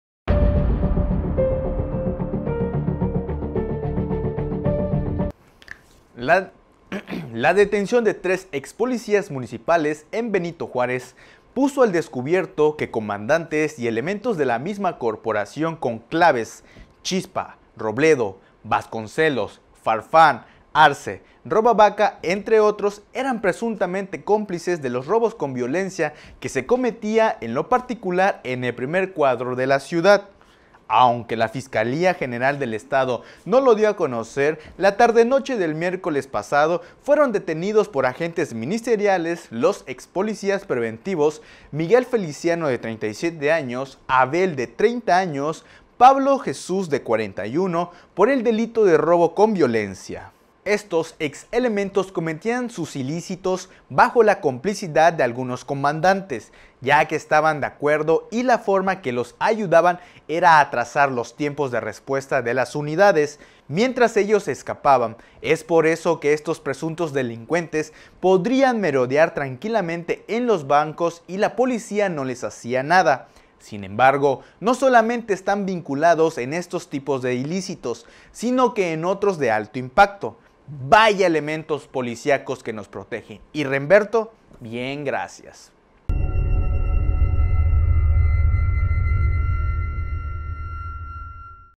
Ex policías y policías cómplices en ilícitos de todo tipo
Entérate de las noticias “EN CORTO”…Ex policías y policías cómplices en ilícitos de todo tipo...